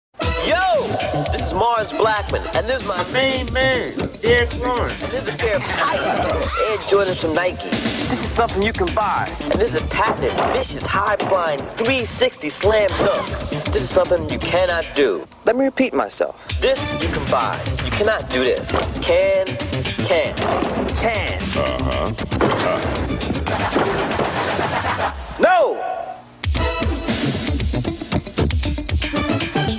Michael Jordan Commercial- Derrick Lawrence Jr.
CBTV Commercial Air Lawrence. Derrick Lawrence Jr. From Clear Brook Highschool Is An Highflying Teenager On His Way To Play College Football In Kansas!!!